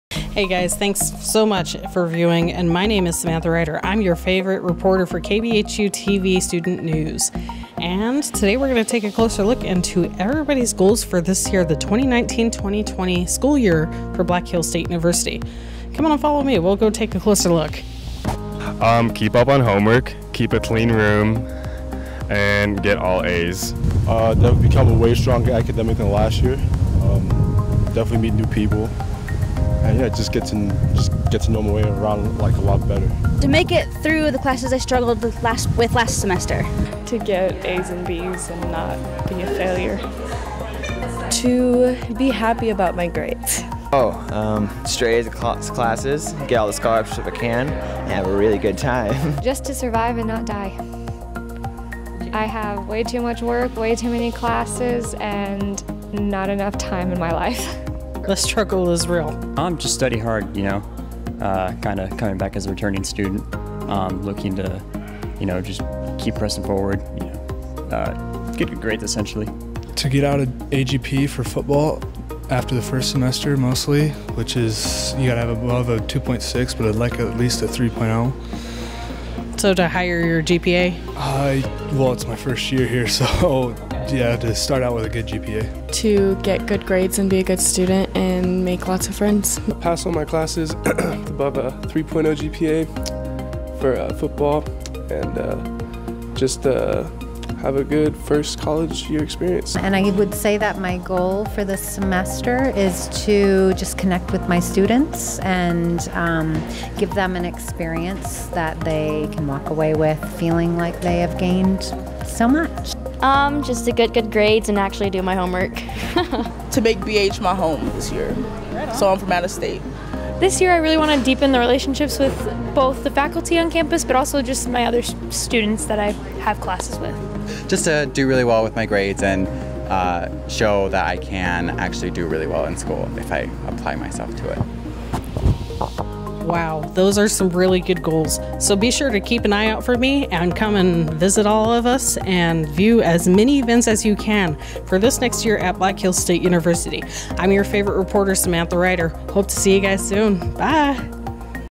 19 20 Goals
Students from Black Hills State University share their goals for the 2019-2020 school year. Filmed and Edited by Samantha Rider.